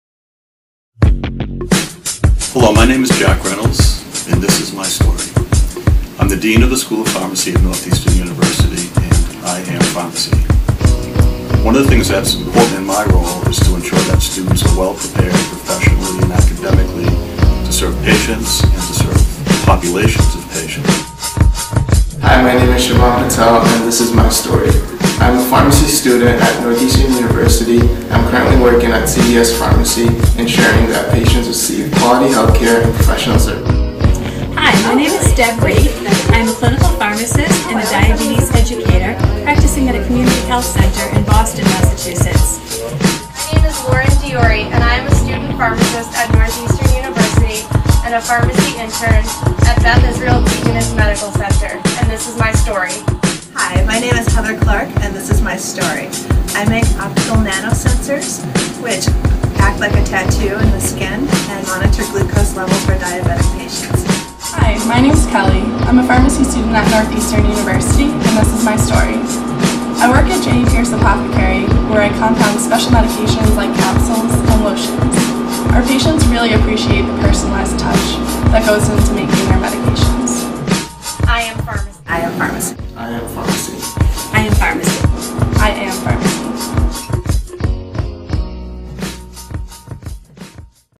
Northeastern University 2012 PharmFlix - Share Your Story
Northeastern University APhA-ASP 2011/2012 PharmFlix  - Share Your Story

Starring: Dean Jack Reynolds, Dr. Debra Reid, Dr. Heather Clark, Shivam Patel, Lauren Diore and Kelly Lynch

Edited by: Deena Magdy.

Directed by: Lucy West and Gabriella Pepe